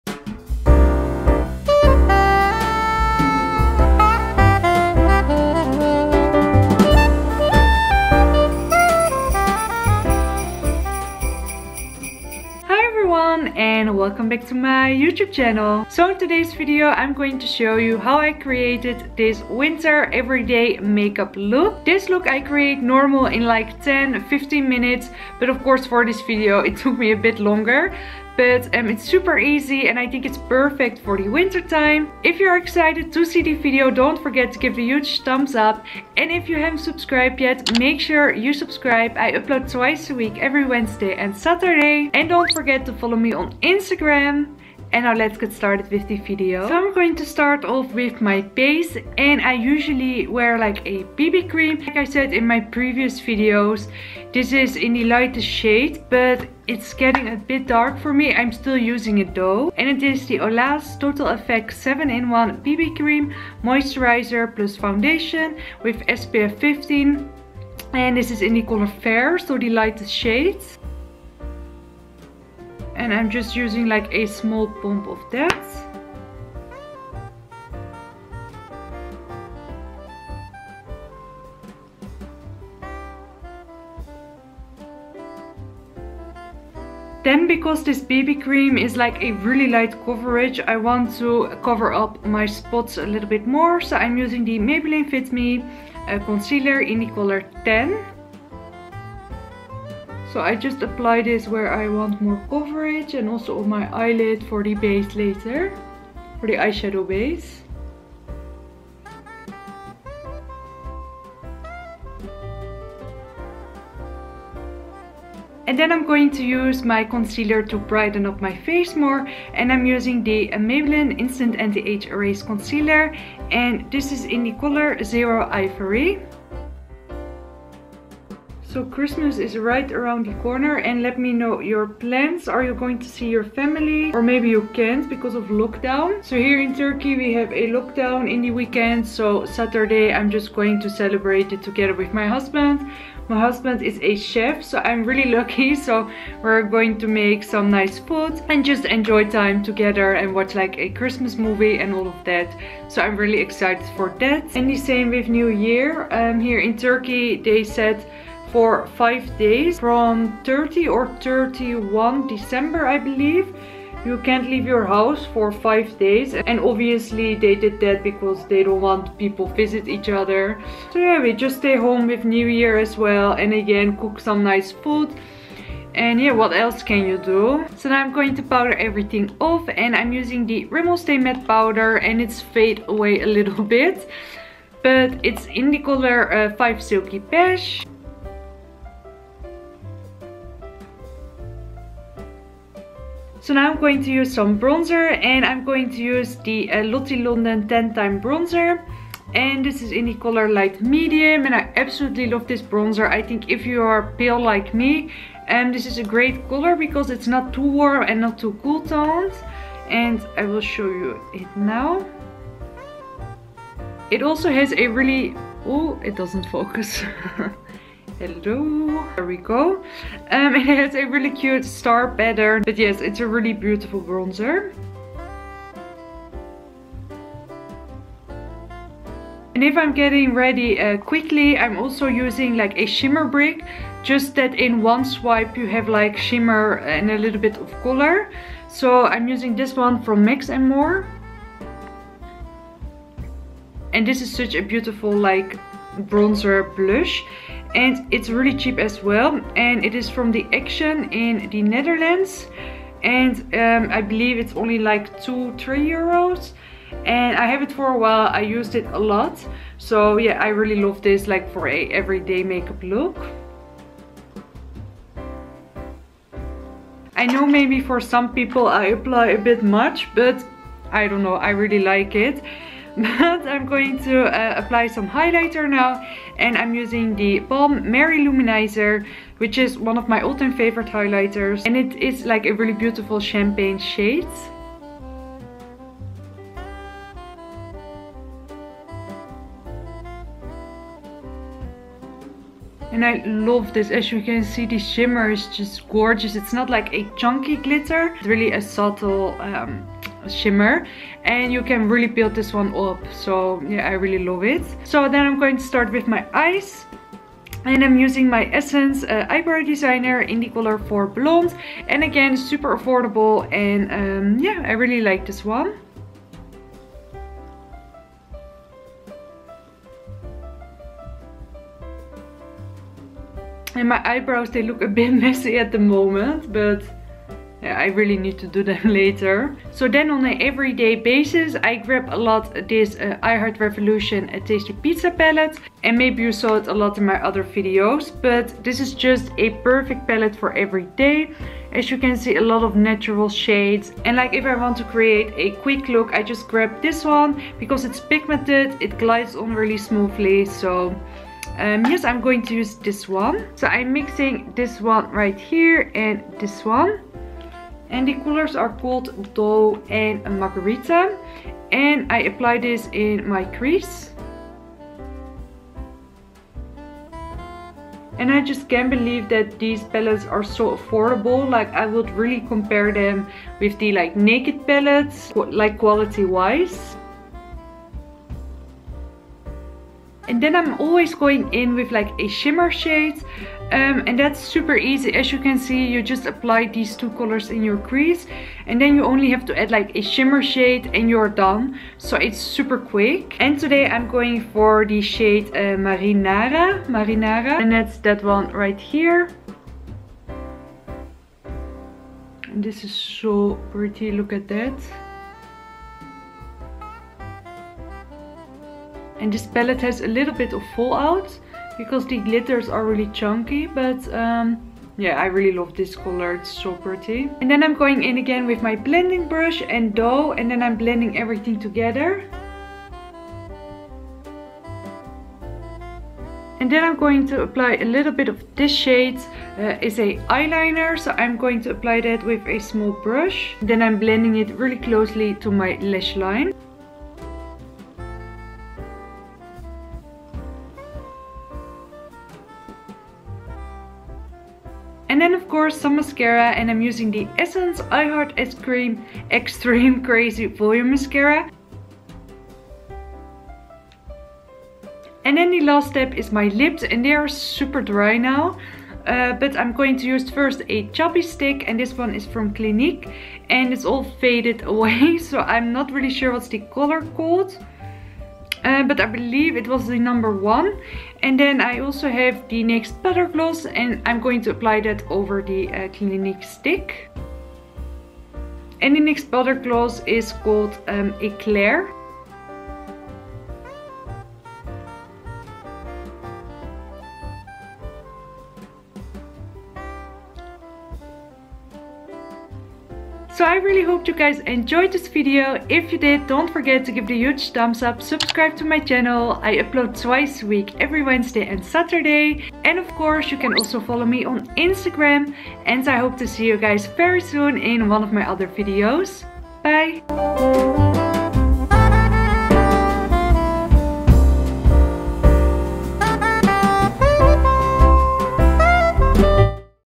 EVERYDAY WINTER MAKEUP ROUTINE❄ALL DRUGSTORE PRODUCTS | My Pretty Everything
Hi loves!💕 Today's video is my winter every day makeup routine, I hope you enjoy it!🎄
• Olaz total effects BB cream fair
• Maybelline fit me concealer 10
• Maybelline anti age eraser concealer
• Rimmel stay matte powder 10
• Lottie London tan time bronzer light/medium
• Max&More Shimmer brick
• The balm marylou minizer highlighter
• Essence eyebrow designer 04 blonde
• I heart revolution tasty pizza eyeshadow palette
• Essence I heart crazy volume mascara
• Catrice cubby stick & NYX buttergloss in Eclair